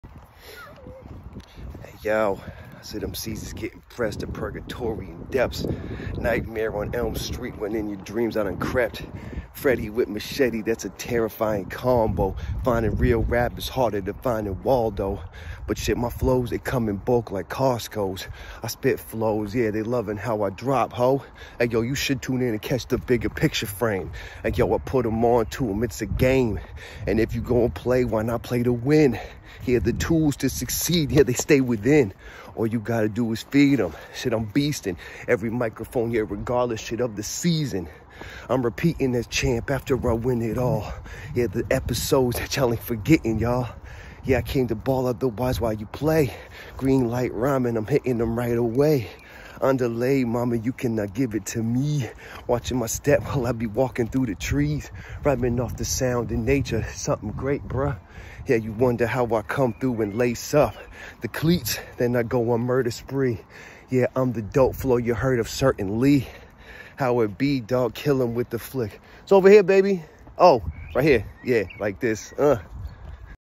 Captains Star Log 5202022
DAILY FREESTYLE 🪐🪐 with a vibe off nature, out here stompin round with mini me had to catch the winds right quick and this is what they whispered for today